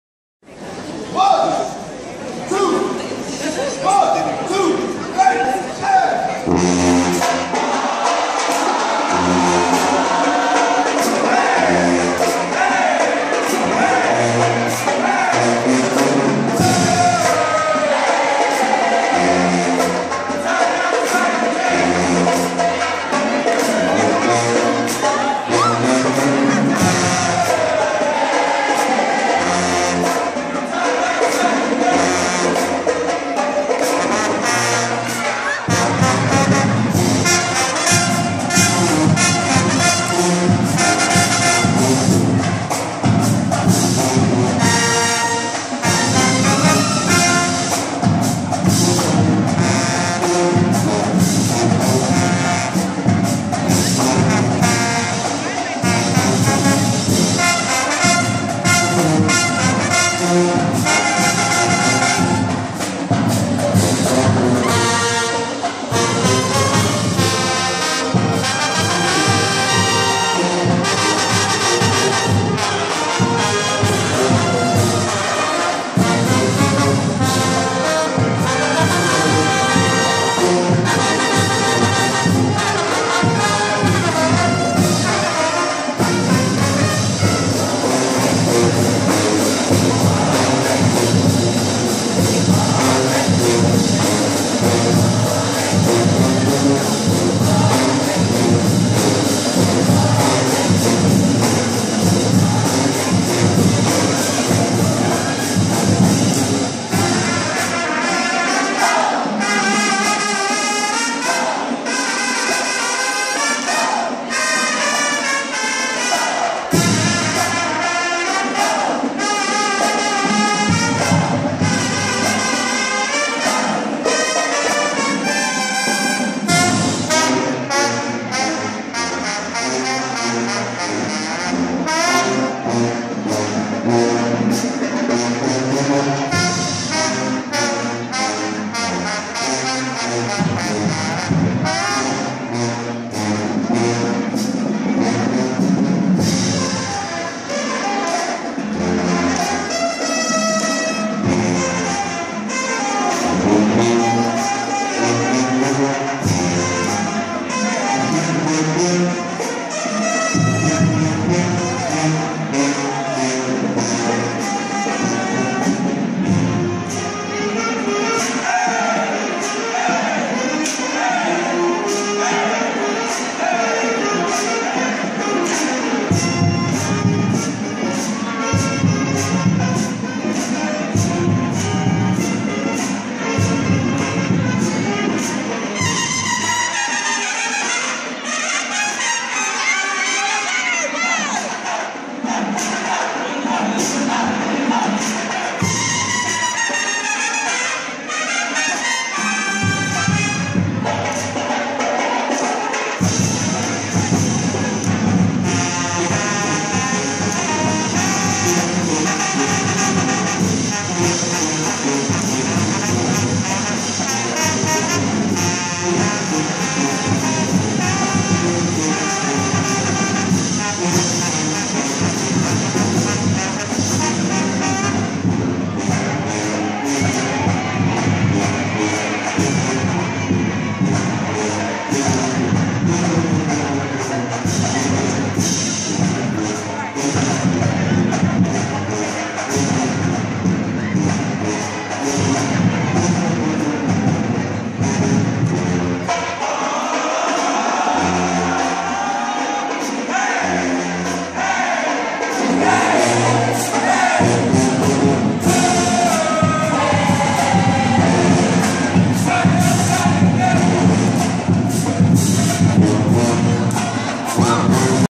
Ooh La La Dancers (2015-2016) - Showtime Marching Band - Howard University - Part 3
Showtime Marching Band performs for the Class of 2019 at the Taste of Howard 2015.